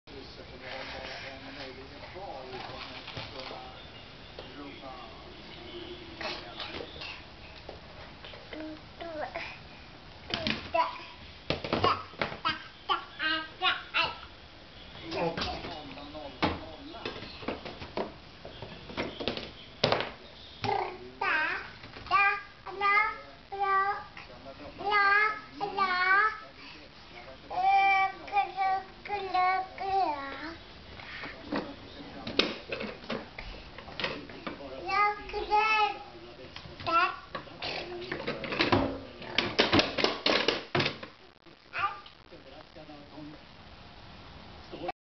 Alvina 16 months
My daughter Alvina playing at home